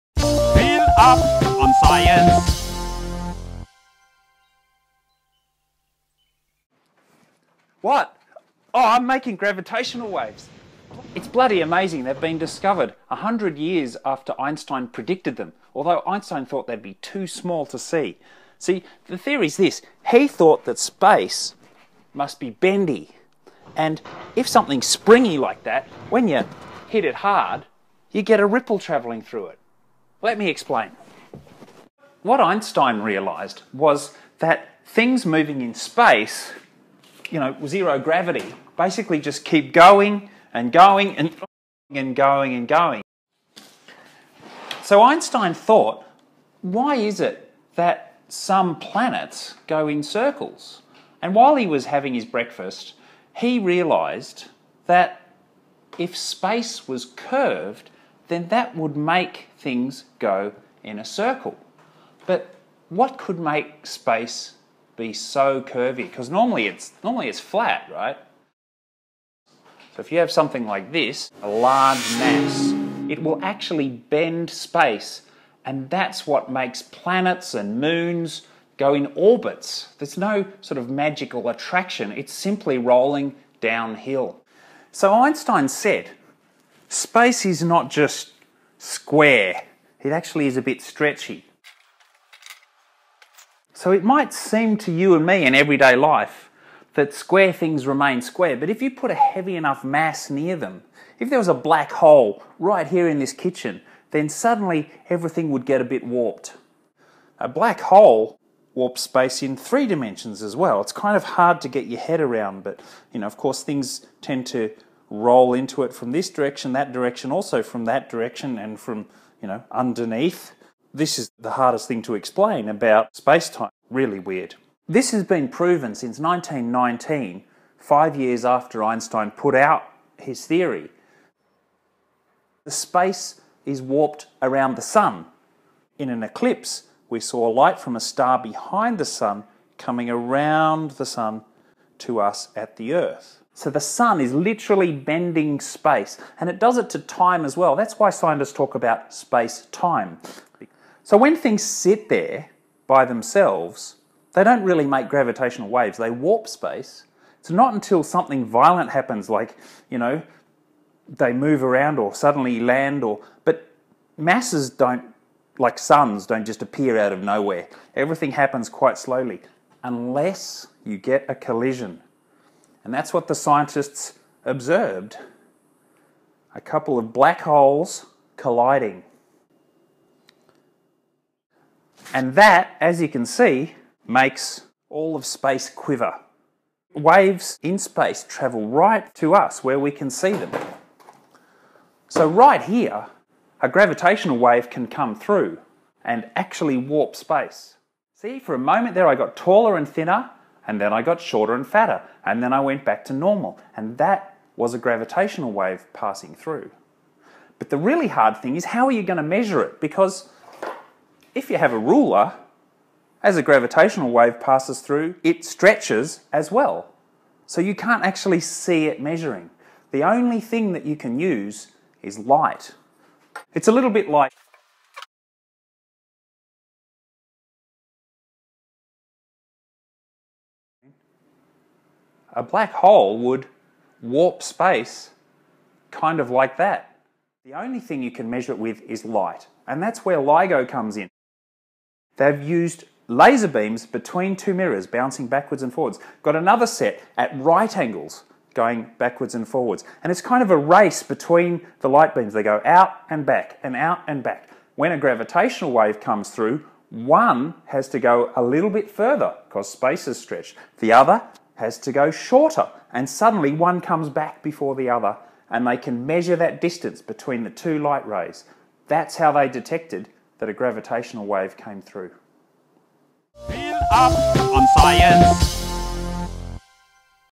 Gravitational waves explained with everyday items
What are gravitational waves, and what happens if you get a black hole in your kitchen?

Einstein thought space was curved, like a tea towel or a cereal bowl.

Dr Phil explains gravitational waves by jumping on the bed and eating breakfast, and black holes with a vase of water.

TRANSCRIPT
Gravitational Waves video transcript

What? I'm making gravitational waves. It's bloody amazing, they've been discovered 100 years after Einstein predicted them, although Einstein thought they'd be too small to see.

See, the theory's this, he thought that space must be bendy and if something is springy like that, when you hit it hard, you get a ripple travelling through it. Let me explain.

What Einstein realised was that things moving in space, you know, zero gravity, basically just keep going and going and going and going and going. So Einstein thought, why is it that some planets go in circles. And while he was having his breakfast, he realised that if space was curved, then that would make things go in a circle.

But what could make space be so curvy? Because normally it's flat, right? If you have something like this, a large mass, it will actually bend space, and that's what make planets and moons go in orbits. It's no sort of magical attraction, it's simply rolling downhill.

So Einstein said, space is not just square, it actually is a bit stretchy. So it might seem to you and me in every day life that square things remain square, but if you put a heavy enough mass near them - if there was a black hole right here in this kitchen then suddenly, everything would get a bit warped. A black hole warps space in 3 dimensions as well; it's kind of hard to get your head around, but, you know of course things tend to roll into it from this direction, that direction, also from that direction and you know, underneath. This is the hardest thing to explain about spacetime - really weird.

This has been proven since 1919, 5 years after Einstein put out his theory. The space is warped around the Sun, in an eclipse we saw light from a star behind the Sun coming around the Sun to us at the Earth. So the Sun is literally bending space, and it's doing it to time as well; that's why scientists talk about spacetime.

So when things sit there by themselves, they don't really make gravitational waves. They warp space, but it's not until something violent happens, like you know, they move around, or suddenly land - but masses like suns don't just appear out of nowhere. Everything happens quite slowly.

Unless you get a collision. And that's what the scientists observed - a couple of black holes colliding. And that, as you can see, makes all of space quiver. Waves in space travel right to us where we can see them.

So right here, a gravitational wave can come through and actually warp space. See, for a moment there I got taller and thinner and then I got shorter and fatter and then I went back to normal, and that was a gravitational wave passing through.

But the really hard thing is how are you going to measure it? Because if you have a ruler, as a gravitational wave passes through, it stretches as well, so you can't actually see it measuring.

The only thing you can use is light. It's a little bit like: a black hole would warp space kind of like that.

The only thing you can measure it with is light, and that's where LIGO comes in. They've used laser beams between two mirrors, bouncing backwards and forwards, got another set at right angles, going backwards and forwards, and it's kind of a race between the light beams: they go out and back, out and back, when a gravitational wave comes through, one has to go a little bit further because space has stretched, the other has to go shorter and suddenly one comes back before the other and they can measure that distance between the two light rays. That's how they detected that a gravitational wave came through.